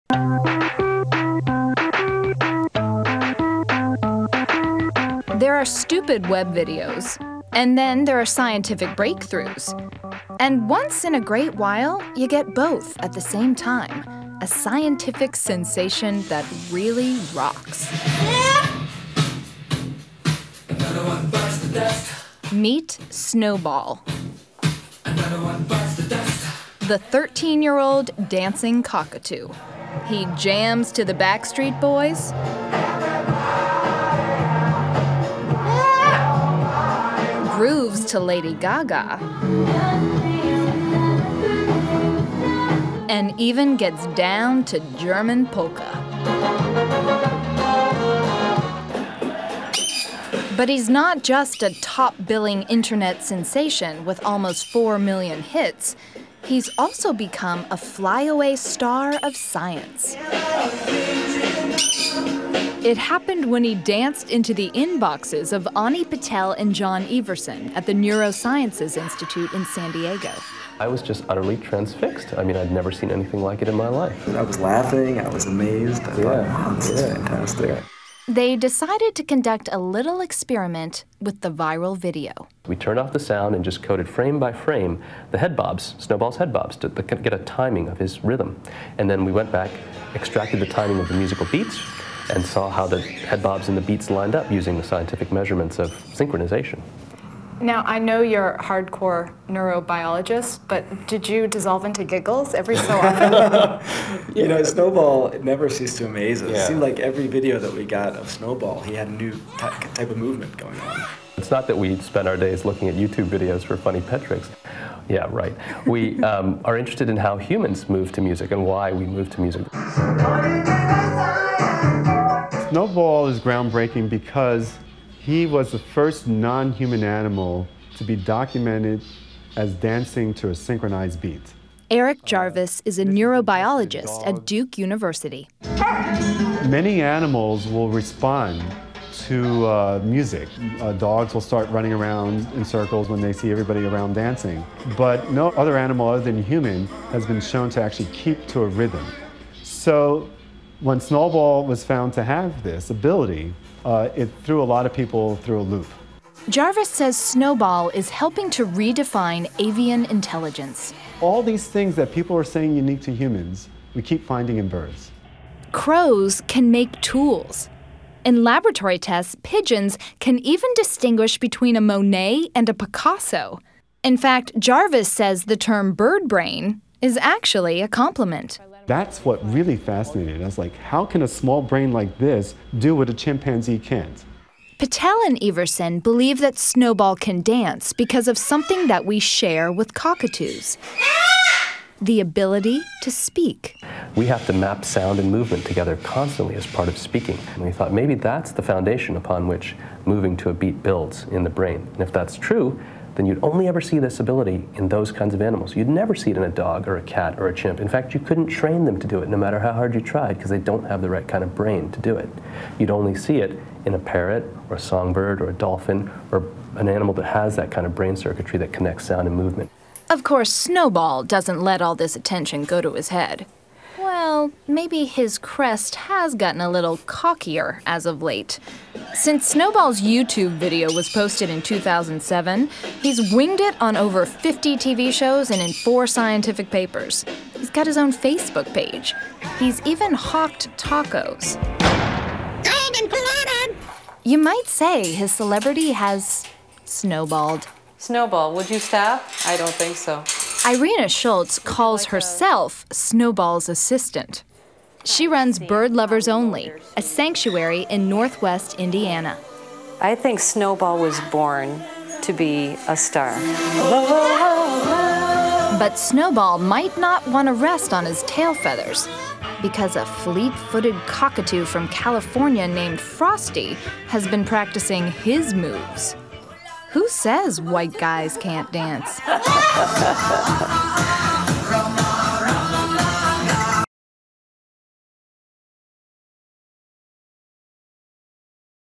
Bird Grooves to the Beat
Meet Snowball, the 13 year-old dancing cockatoo. He jams to the Backstreet Boys and grooves to Lady Gaga. Neurobiologists used scientific measurements to find that Snowball is the first nonhuman animal to be documented as dancing to a synchronized beat.